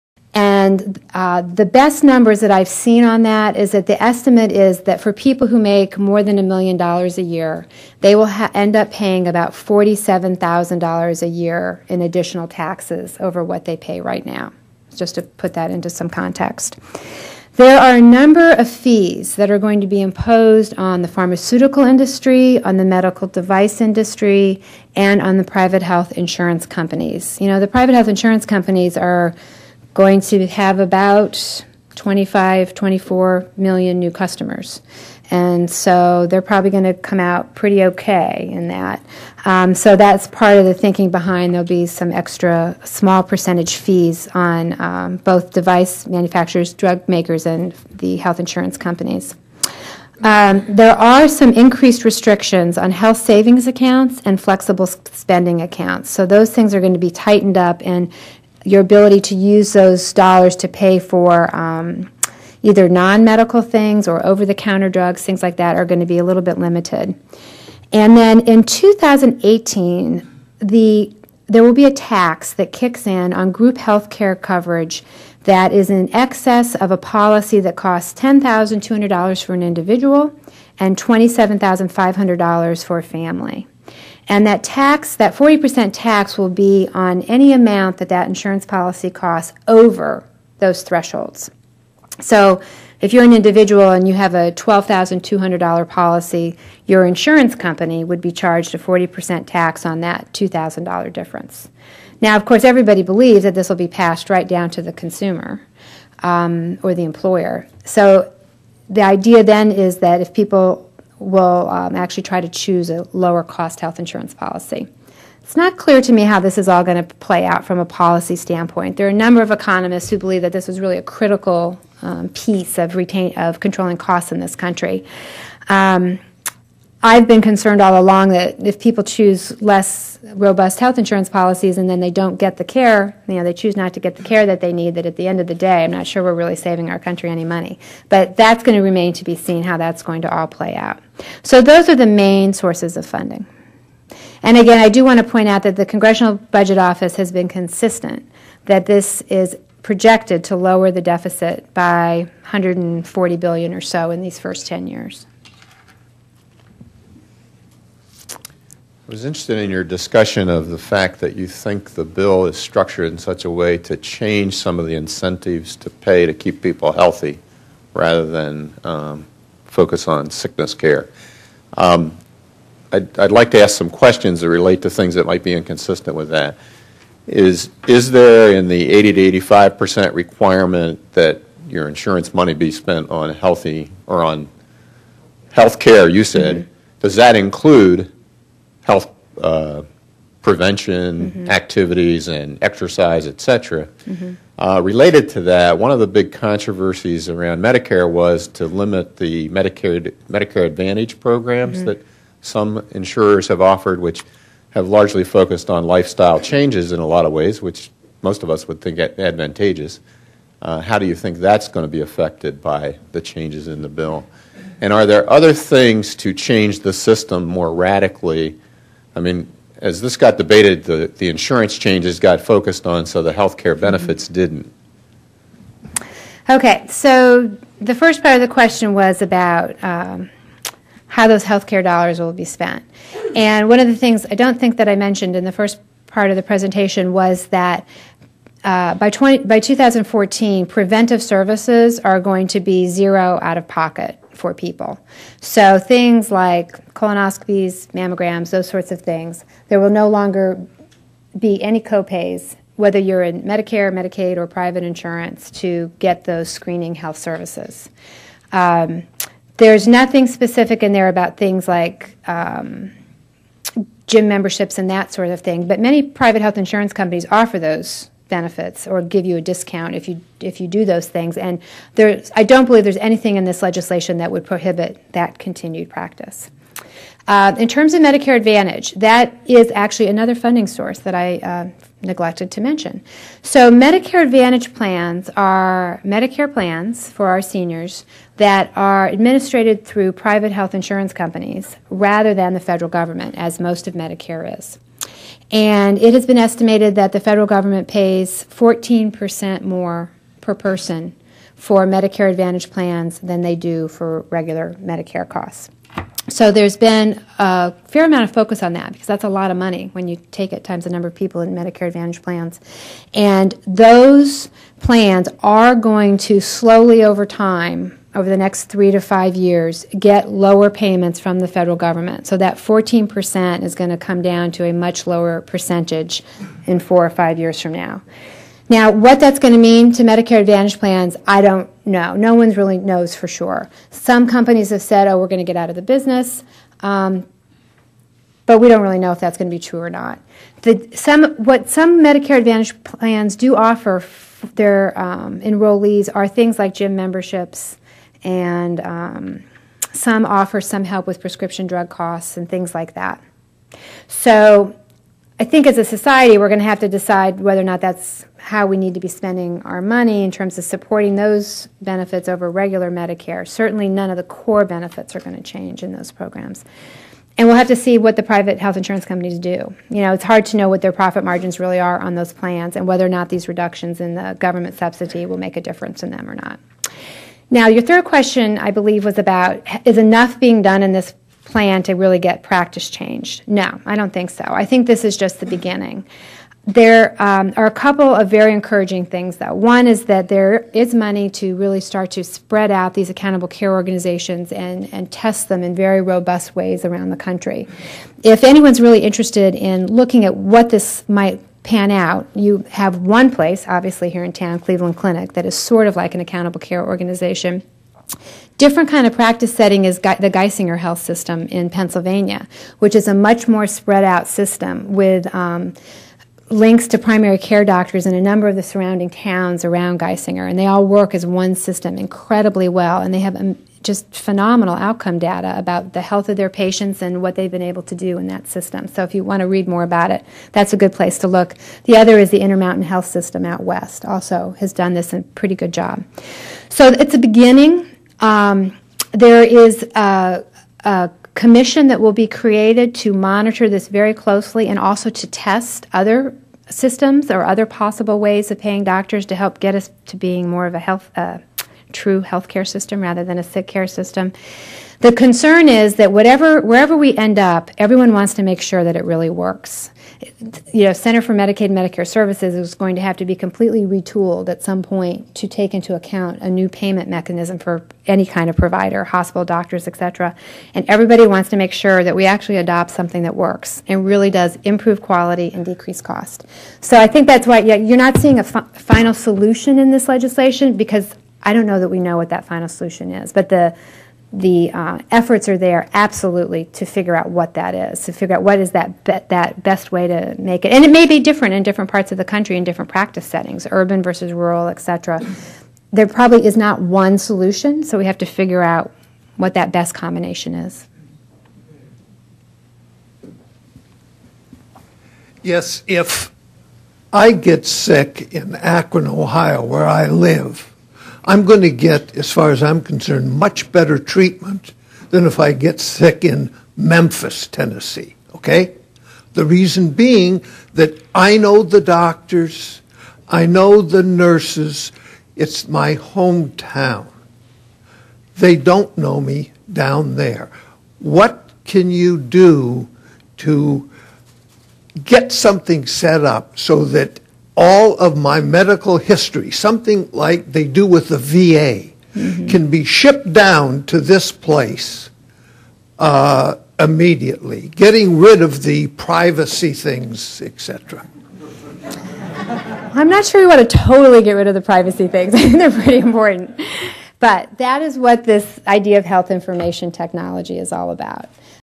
Valerie Arkoosh Pt. 5/6
Valerie A. Arkoosh, MD, MPH, President, National Physicians Alliance, speaks about the focus on comprehensive state-based health care reform efforts and their impact on the uninsured. 4.9.10